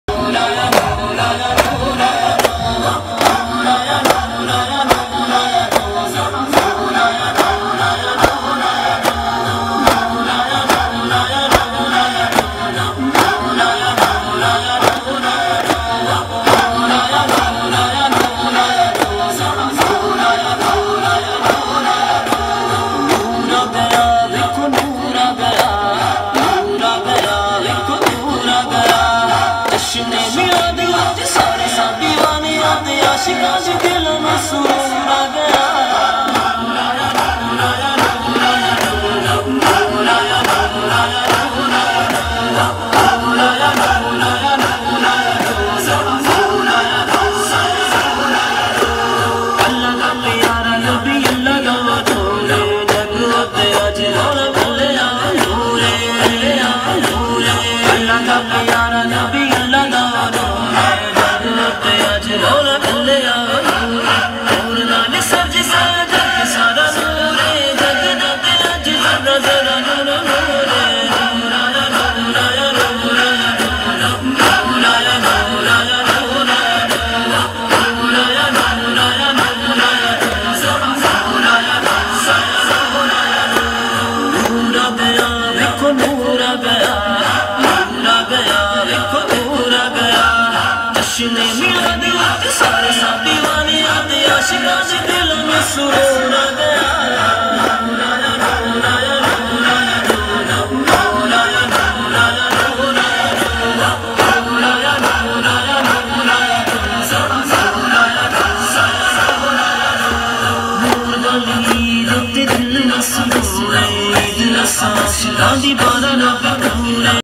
Duff Kottu 2019- Amazing Duff Kottu MJMC Charmadi
Amazing Duff kottu step by Charmadi islamic students in the year of 2019.
Place of duff kottu - Beetige(silicon city)
Videography 📼 Top Gear Beetige
Starring-
Beetige friends
Best wishes from
Friends Beetige & UP family